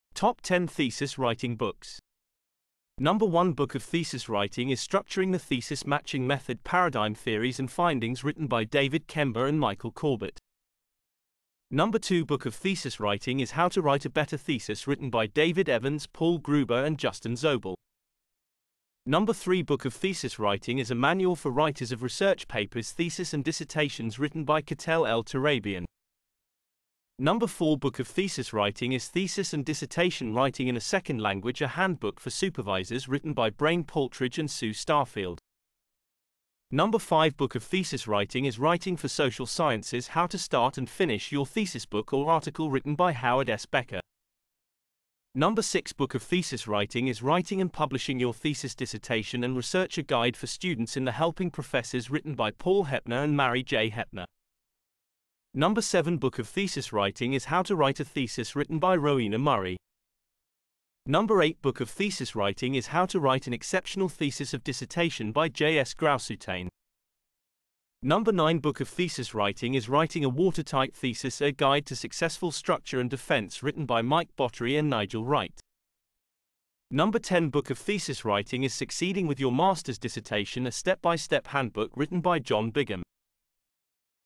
Top 10 Thesis Writing Books | How to Write a Thesis ?
This video is about top 10 books of thesis books which will help you how to write thesis /dissertations, books, articles ...etc. Watch till end.

how to write thesis,
thesis write up,
How to write a dissertation or thesis,
top 10 thesis writing books,
thesis writing books